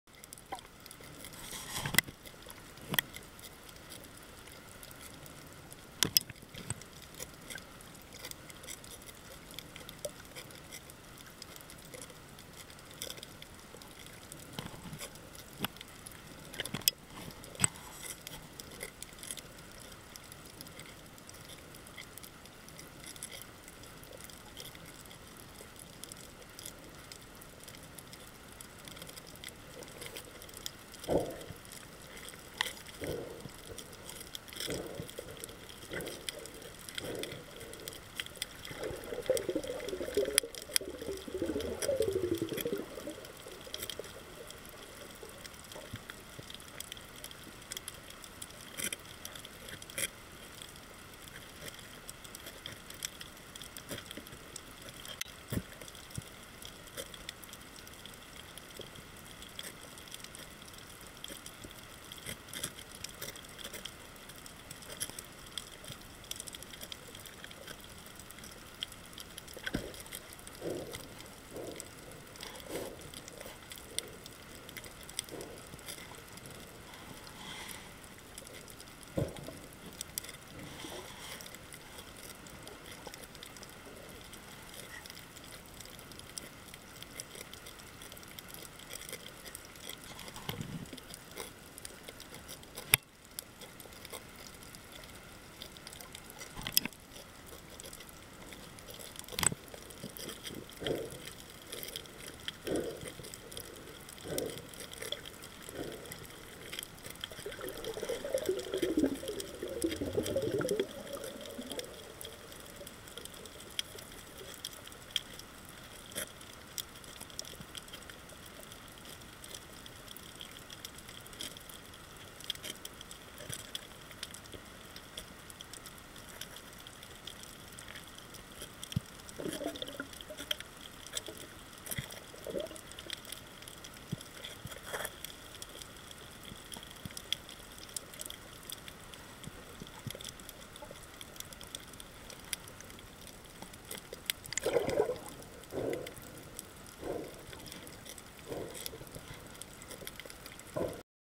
Parrotfish in Hens and Chickens Reef
Filmed these parrotfishes in Hens and Chickens Reef. Will post a another video soon.